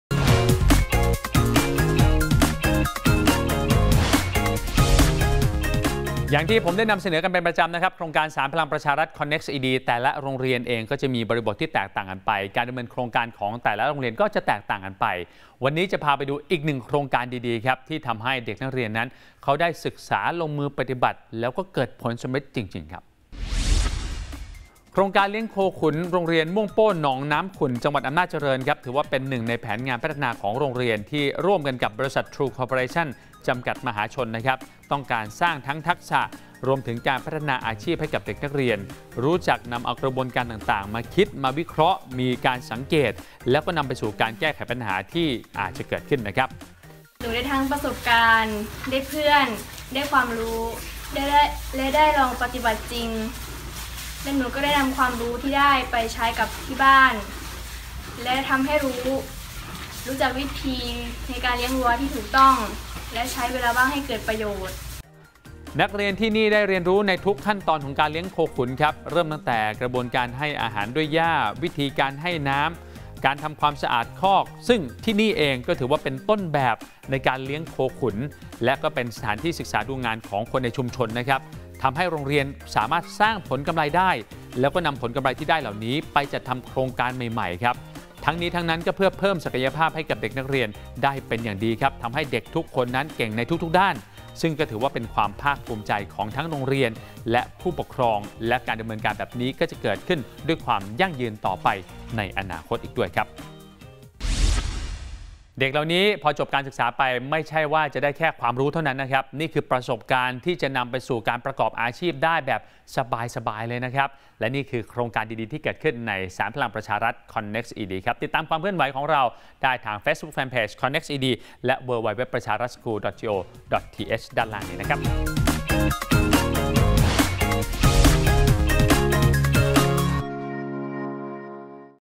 ::05/08/61 | TRUE :: “โครงการเลี้ยงโคขุน” รร.ม่วงโป้หนองน้ำขุ่น จ.อำนาจเจริญ
CONNEXT ED NEWS – การเรียนรู้นอกห้องเรียนเป็นการเพิ่มทักษะด้านประสบการณ์ให้กับเด็กนักเรียนได้เป็นอย่างดี ติดตามในช่วง สานพลังประชารัฐ CONNEXT ED NEWS (คอนเน็กซ์อีดี นิวส์)

โครงการเลี้ยงโคขุน โรงเรียนม่วงโป้หนองน้ำขุ่น จ.อำนาจเจริญ หนึ่งในแผนพัฒนาของโรงเรียนร่วมกับ บริษัท ทรู คอร์ปอเรชั่น จำกัด(มหาชน) ที่ต้องการสร้างทักษะและพัฒนาอาชีพให้กับนักเรียน รู้จักนำมาใช้ในการคิด วิเคราะห์ สังเกต และแก้ไขปัญหา ที่อาจเกิดขึ้นได้ โดยเด็กนักเรียนจะได้เรียนรู้ในทุกขั้นตอนของการเลี้ยงโคขุน เริ่มจากการให้อาหารด้วยหญ้า วิธีการให้น้ำ รวมถึงการทำความสะอาดคอก ซึ่งสถานที่แห่งนี้ เป็นต้นแบบในการเลี้ยงโคขุน เป็นสถานที่ศึกษาดูงานของชุมชน ทำให้โรงเรียนสามารถนำผลกำไรที่ได้ ไปจัดทำโครงการใหม่ๆ  เพื่อเพิ่มศักยภาพเด็กนักเรียนให้เป็นเด็กดี เด็กเก่งในทุกๆ ด้าน เป็นความภาคูมิใจของโรงเรียน และผู้ปกครอง อย่างยั่งยืนต่อไปในอนาคต

กิจกรรมต่างๆที่เกิดขึ้นในโรงเรียน ล้วนแล้วแต่จะติดตัวเด็กนักเรียนไปเมื่อจบการศึกษา เท่ากับว่าเป็นการจุดประกายการสร้างอาชีพให้กับเด็กๆด้วย สามารถติดตามข่าวสารต่างๆของโครงการสานพลังประชารัฐ CONNEXT ED ได้ที่ facebook CONNEXT ED และทางเว็บไซต์